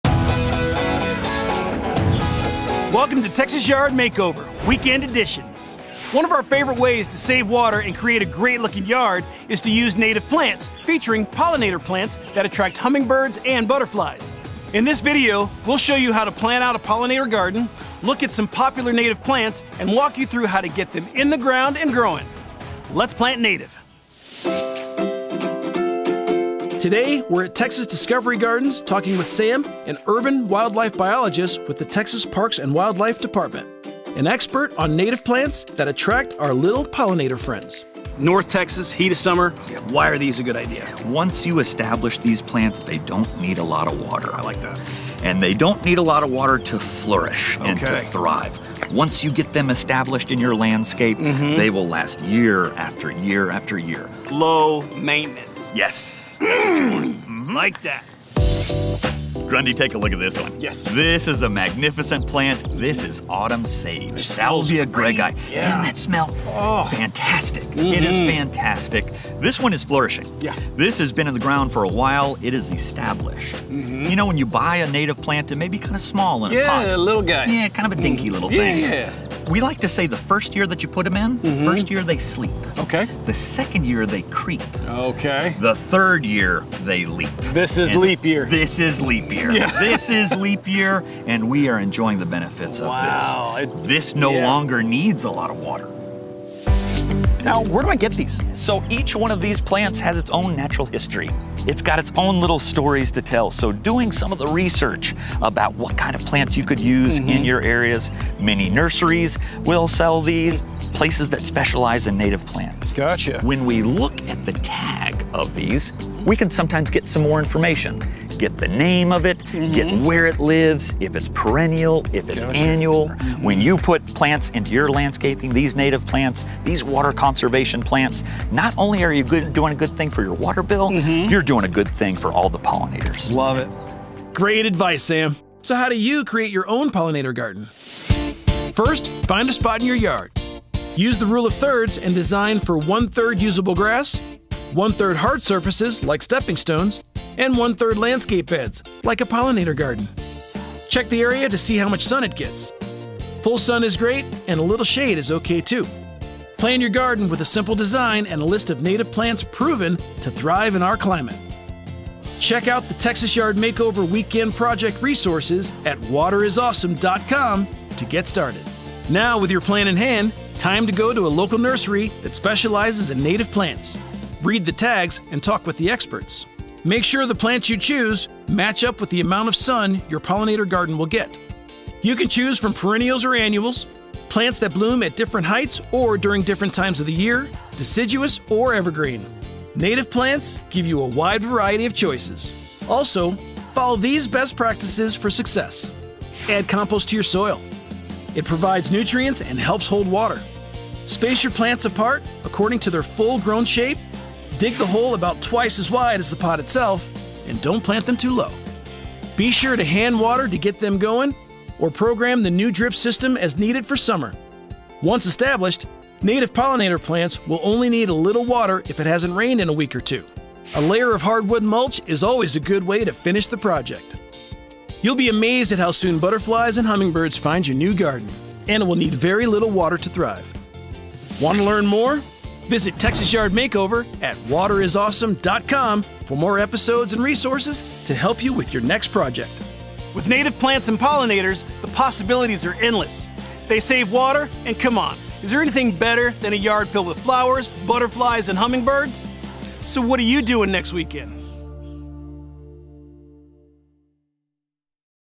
Texas Yard Makeover - Weekend Edition - Create a Pollinator Garden Using Native Plants
On this episode of Texas Yard Makeover - Weekend Edition, we meet up with Sam Kieschnick, an Urban Wildlife Biologist with the Texas Parks and Wildlife Department, at the Texas Discovery Gardens to talk about native plants that attract butterflies and hummingbirds. We'll also show you how to plan out a pollinator garden, look at some popular native plants and walk you through how to get them in the ground and growing. So, what are you doing next weekend?